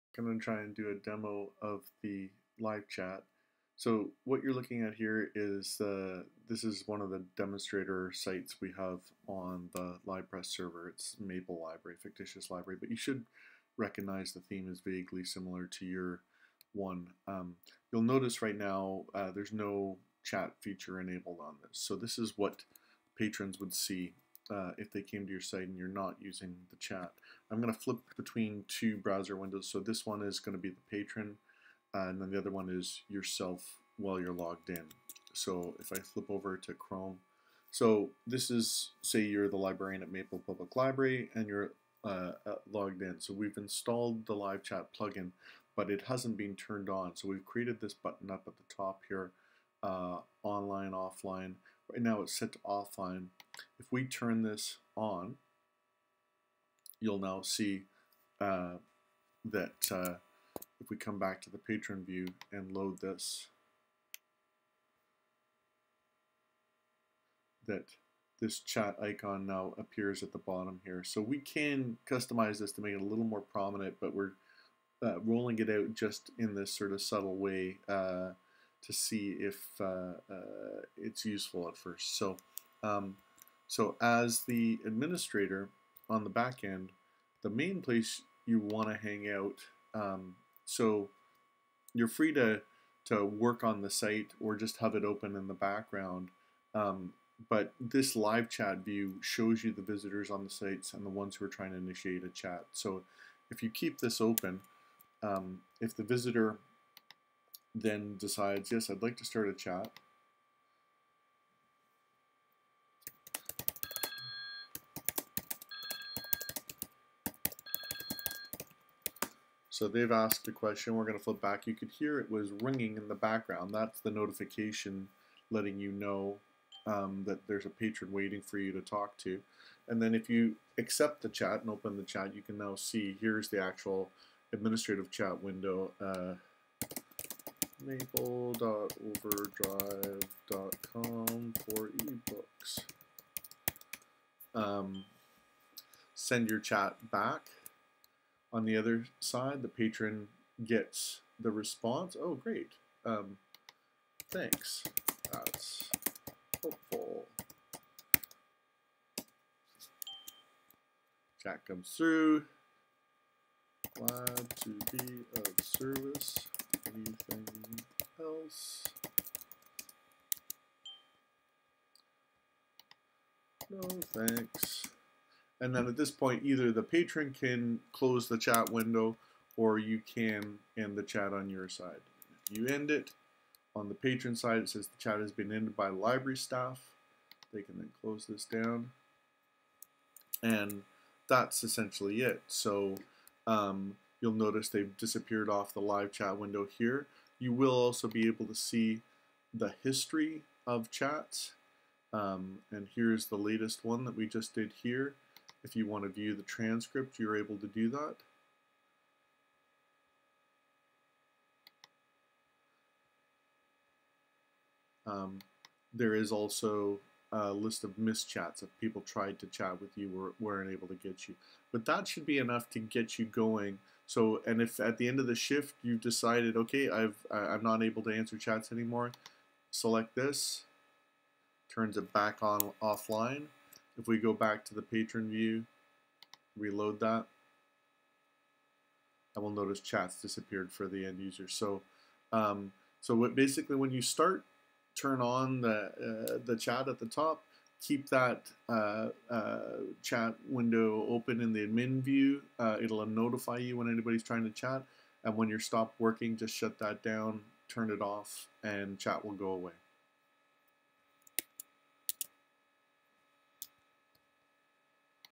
Using Chat on LibPress
short video showing how to use the newly installed chat feature on your LibPress site